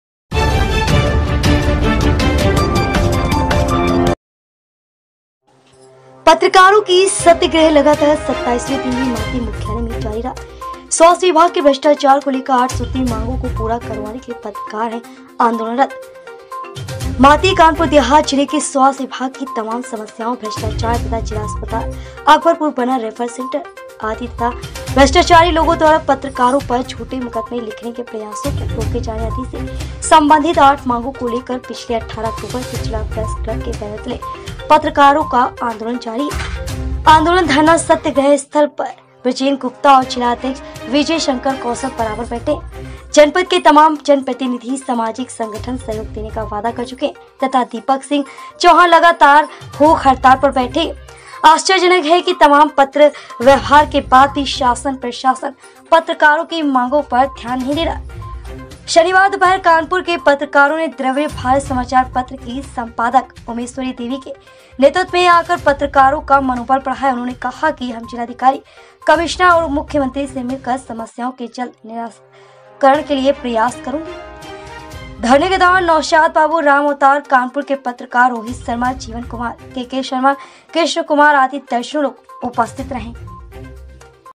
kanpur Dehat । पत्रकारों का सत्याग्रह लगातार 27 वें दिन भी माती मुख्यालय में रहा जारी
कानपुर देहात ,पत्रकारों के सत्याग्रह लगातार 27 वें दिन भी माती मुख्यालय में जारी रहा